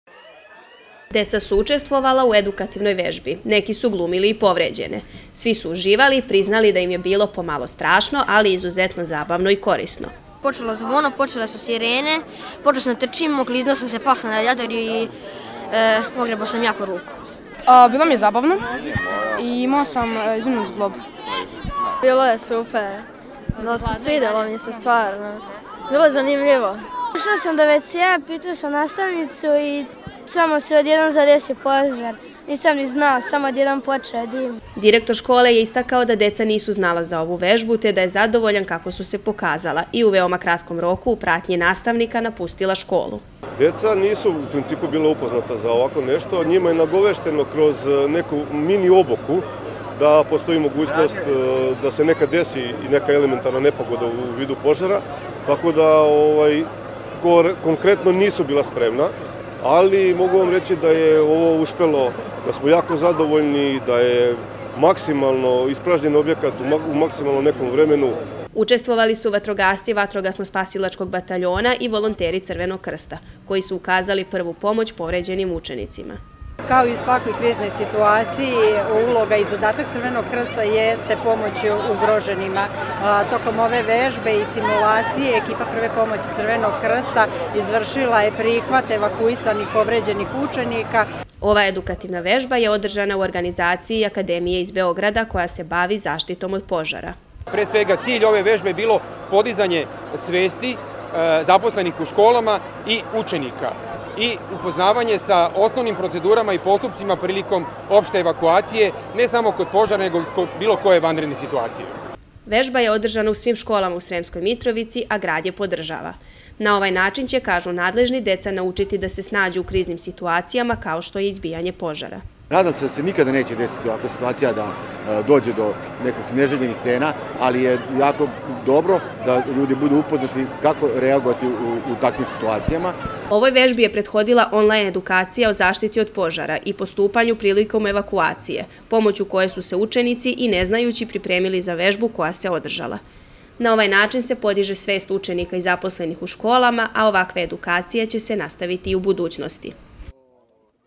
Vežba evakuacije
U Osnovnoj školi "Jovan Popović" u Sremskoj Mitrovici, održana je edukativna vežba evakuacije u kojoj su učestvovala deca i zaposleni, koji su na praktičan način saznali kako se postupa u slučaju izbijanja požara. Organizator ovog događaja je Organizacija koja se bavi zaštitom od požara iz Beograda, a u saradnji sa Gradom Sremska Mitrovica. Ova vežba se održala u svim školama u Mitrovici u cilju podizanja svesti zaposlenih i učenika u školama i učenju kako se postupa u kriznim situacijama.